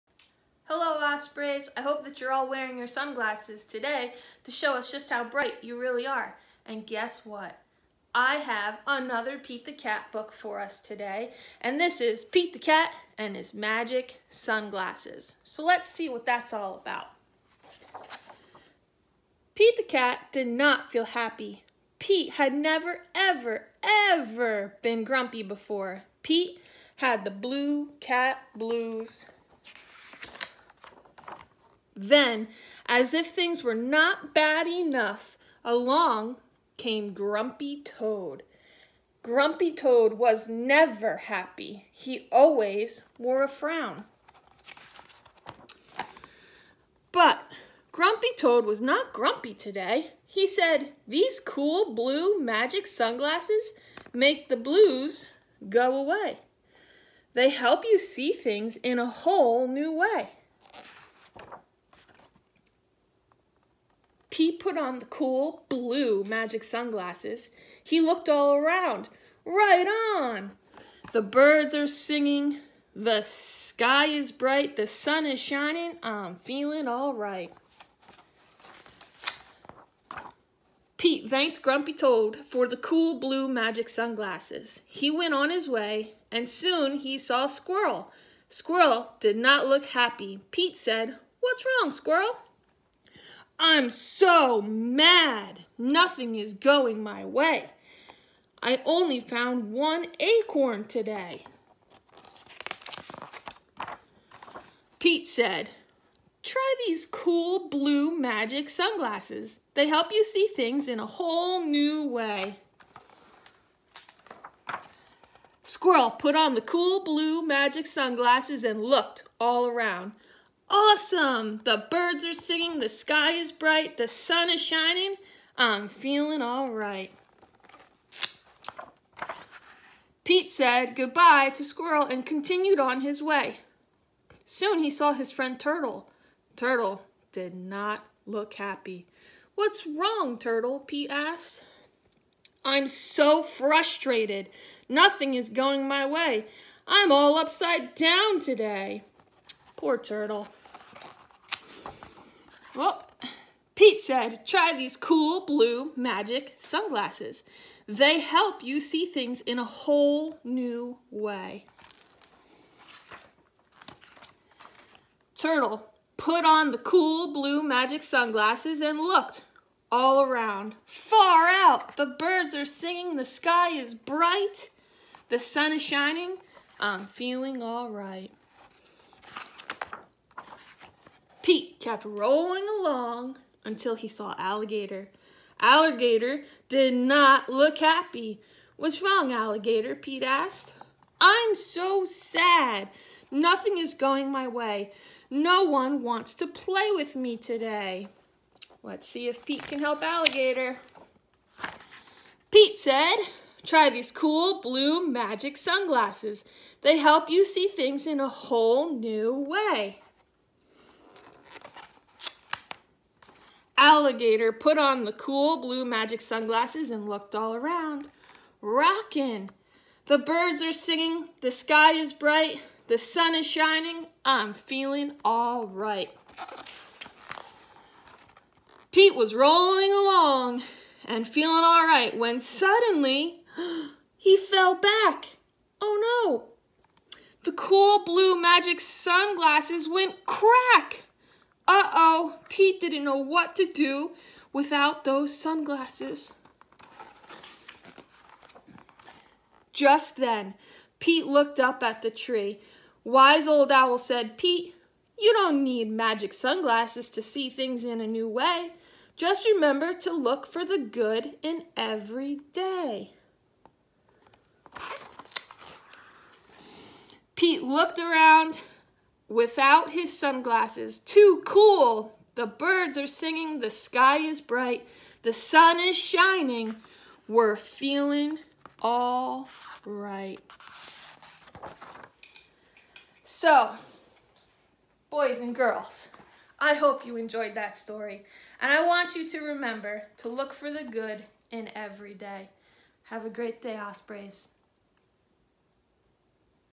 Pete the Cat and the Magic Sunglasses
Ms. Munn shares with us another fun Pete the Cat book! Remember to always look for the good in every day!